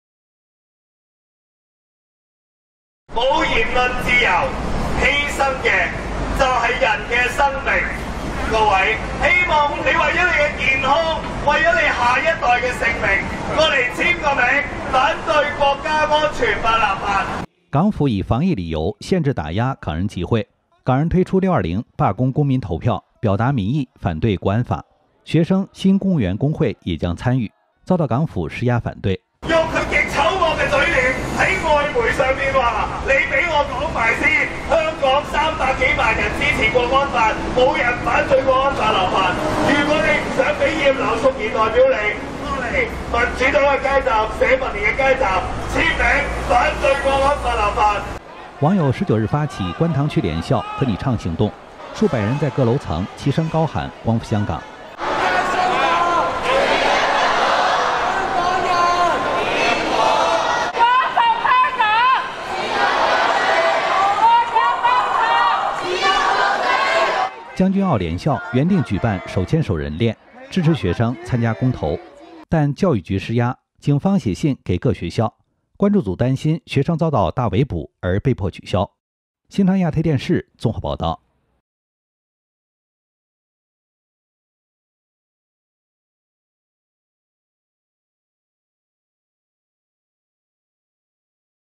國安法或三天通過 620港罷工罷課公投抗爭｜新唐人亞太電視｜20200620
中共人大常委18日起，審議港版國安法，並且不排除在3天會期內通過。而港人持續抗爭，將在6月20日，發起罷工罷課，公民投票展現民意的活動，港府不斷施壓，阻擋公投。